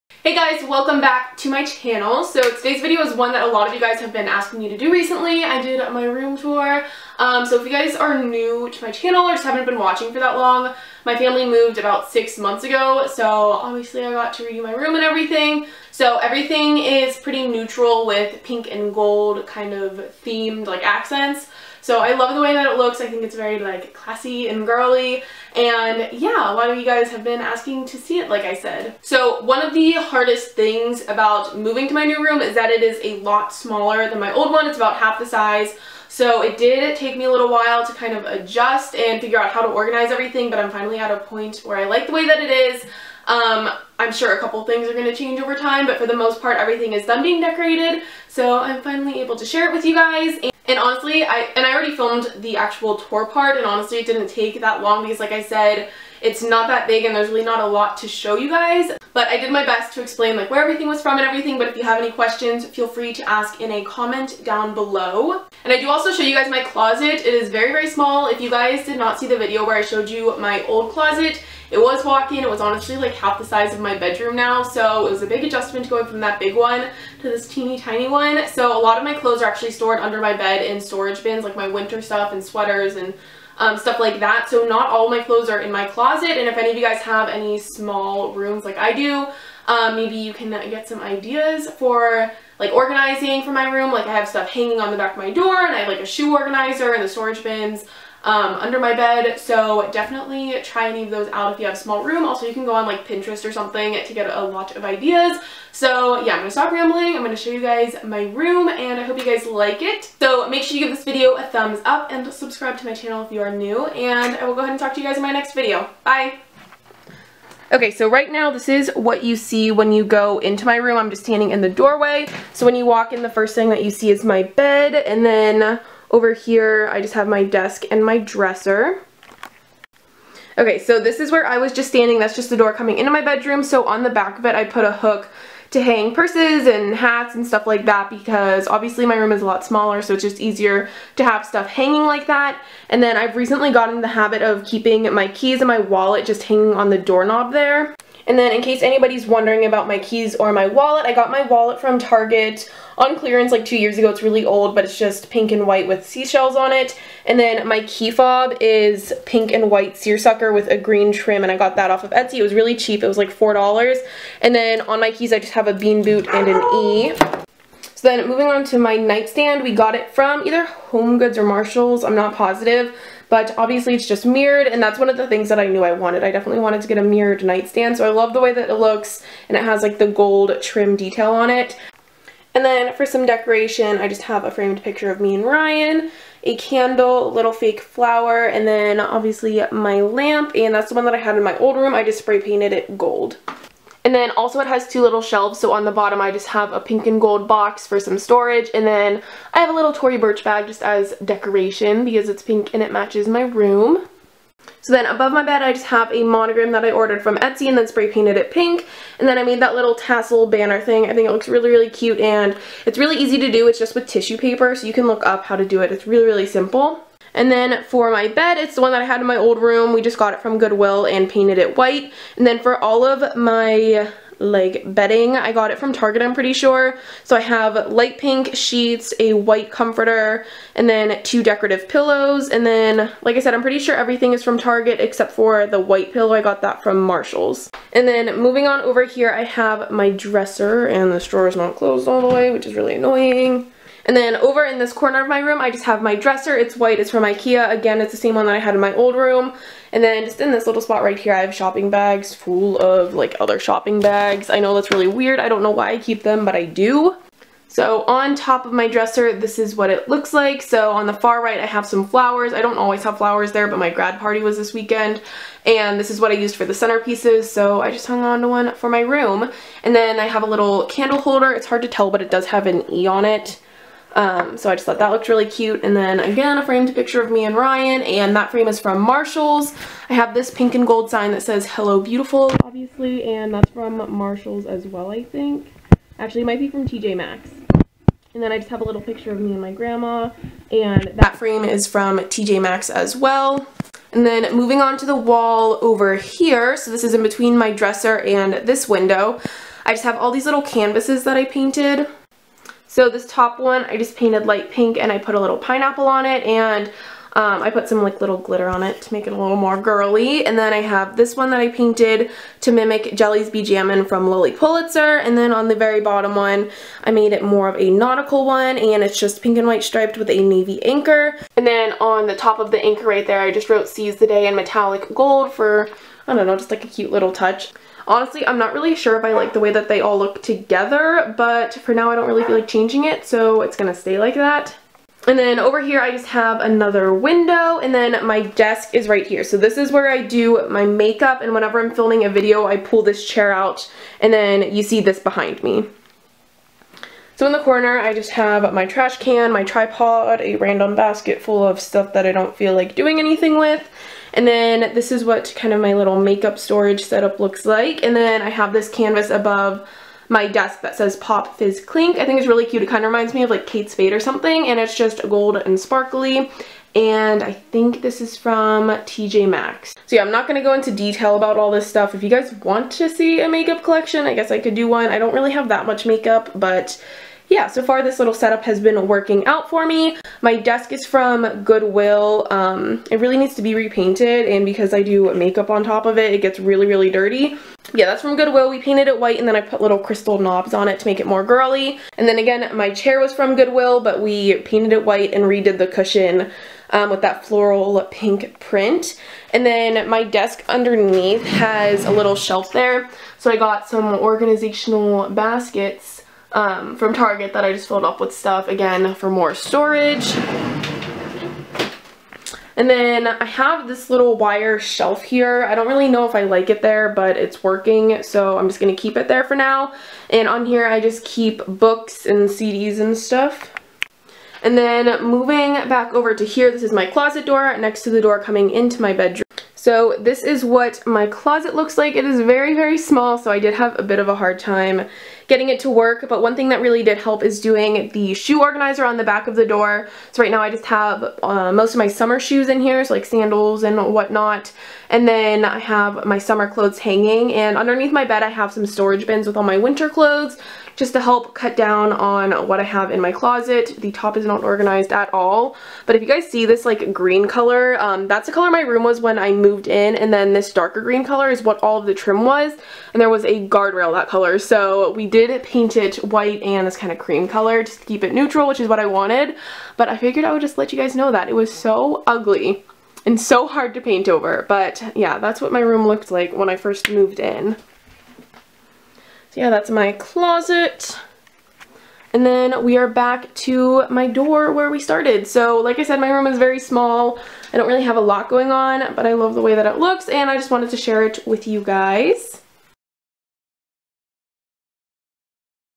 ROOM TOUR! Pink and Gold Theme
I know a lot of you guys have been eaiting for this video, so I hope you like it! I'm really happy with the way it turned out and I hope it helps some of you out with decor ideas or organization!

Wooden Monogram